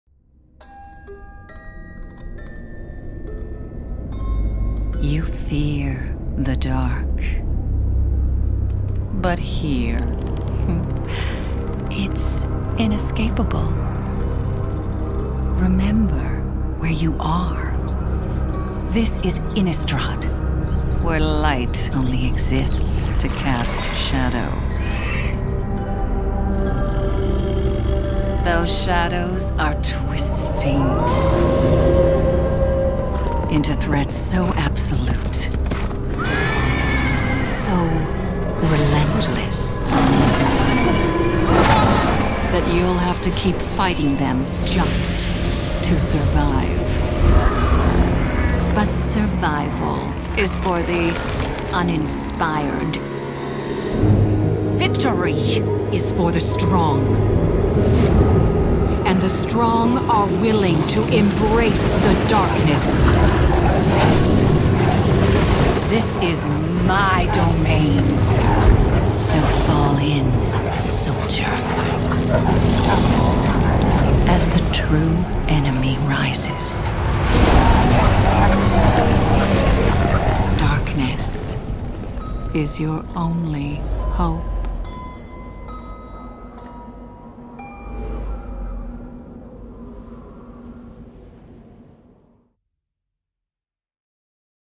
Düstermond-Trailer
Im Angesicht der wahren Quelle des Wahnsinns auf Innistrad, ruht alle Hoffnung dieser Welt plötzlich auf einer unerwarteten Verbündeten. Stellt euch der Dunkelheit – beim Prerelease am 16. und 17. Juli 2016. Düstermond erscheint am 22. Juli.